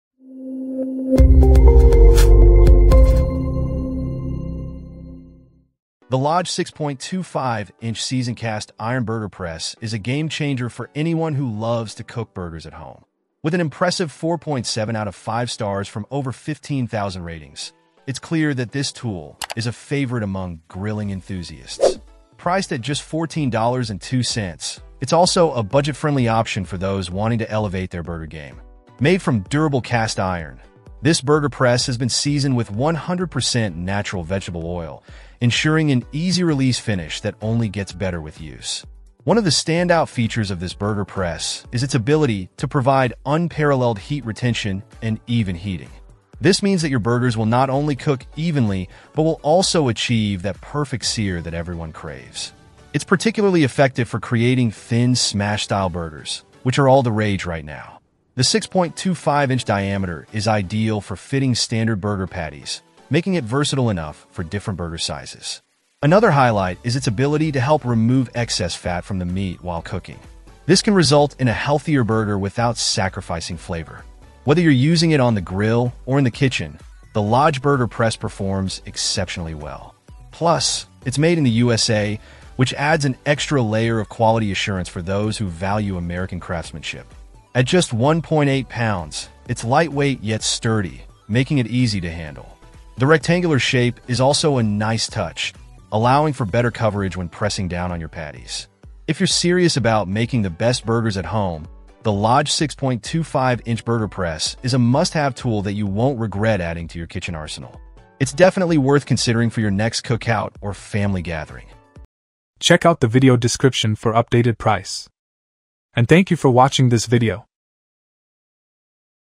Lodge 6.25" Burger Press: The Best Tool for Perfect Burgers
0:00 Intro
0:07 Review

Things that we mentioned in this video:
Lodge 6.25" Burger Press, Cast : B0971N13D3